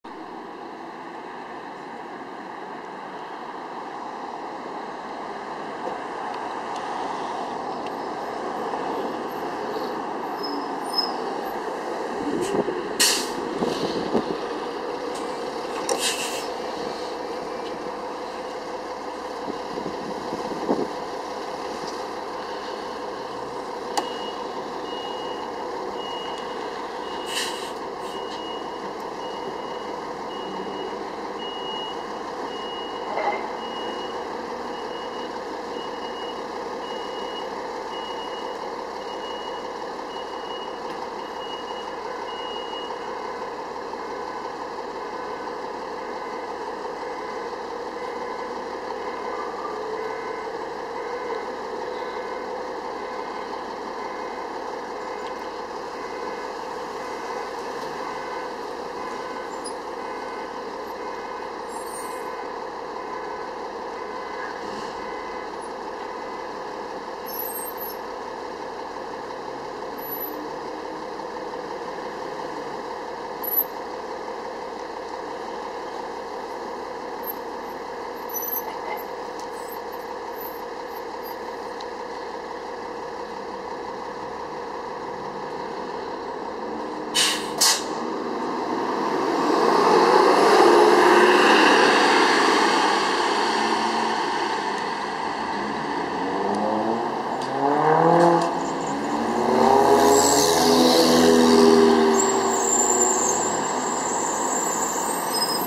MTA New York City Bus 2011 New Flyer XD40 "Xcelsior" 4819 On The Q27 @ Hillside Ave/Springfield Blvd
Seen here is New Flyer XD40 "Xcelsior" 4819 operating on the Q27 to Flushing - Main Street arriving & departing from the Springfield Blvd & Hillside Avenue bus stop